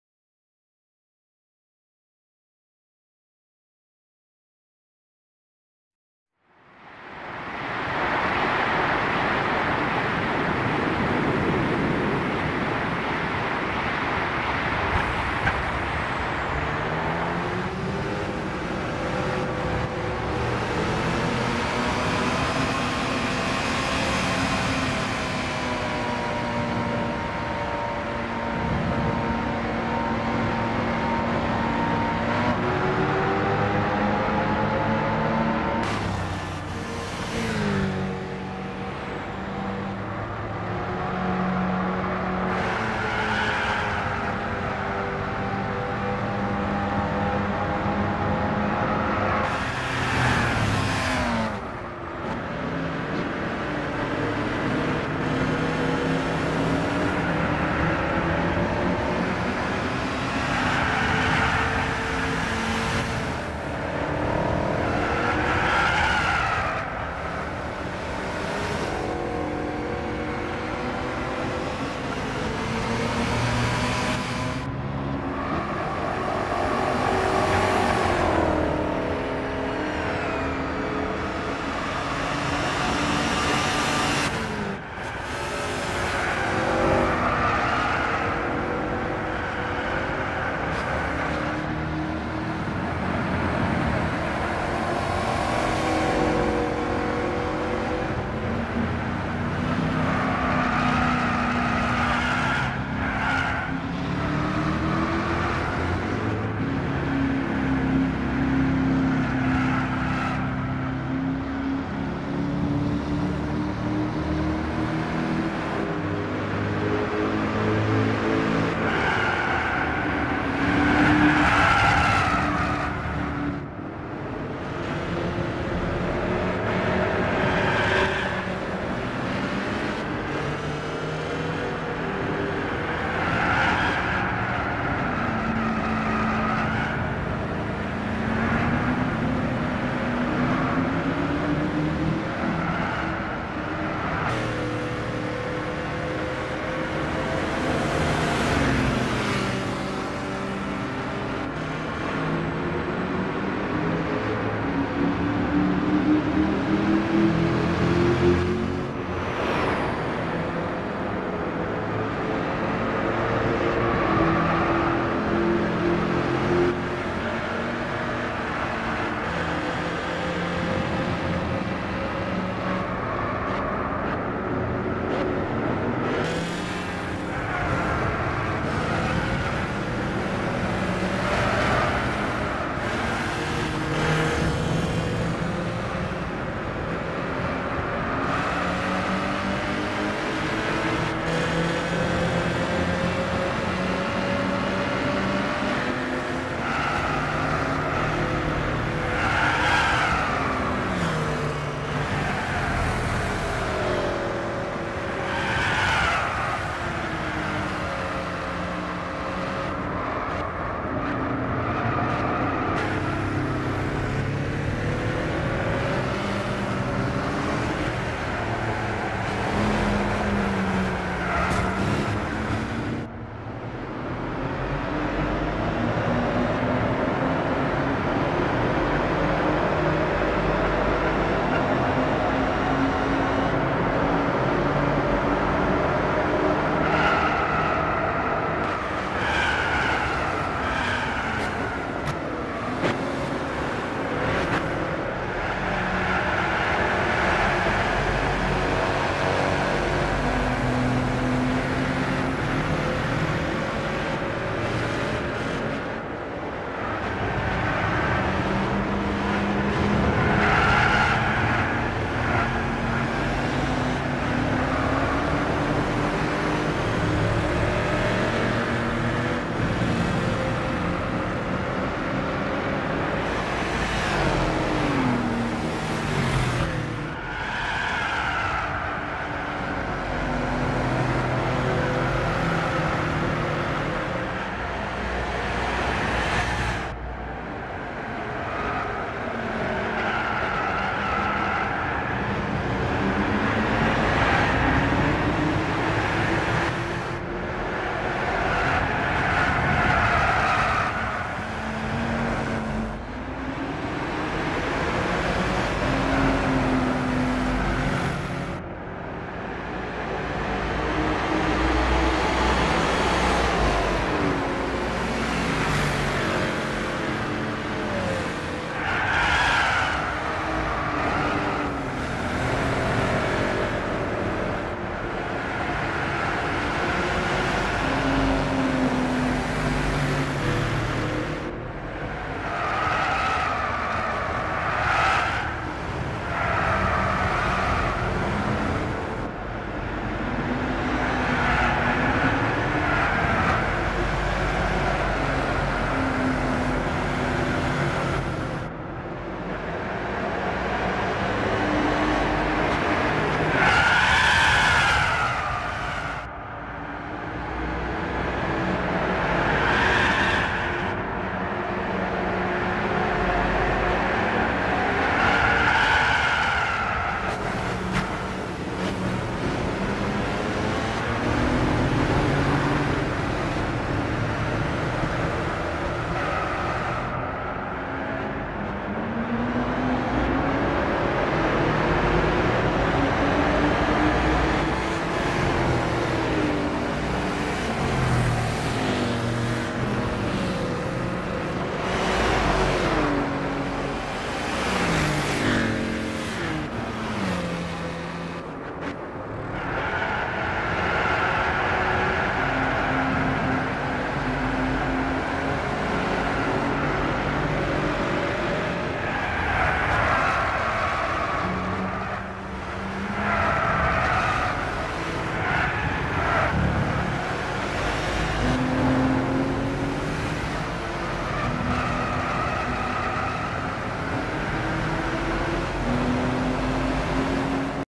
Seasonal Allergies - '12 Tesla Model S Signature Performance 15th Anniversary Edition - Silverstone
I'm used to listening to my tyres, engine and gear changes during a race, but I found the lack of an engine note disturbing.  Not bad entirely, but definitely different than what I'm used to.

Almost like driving in outer space... until the tyres start screaming again.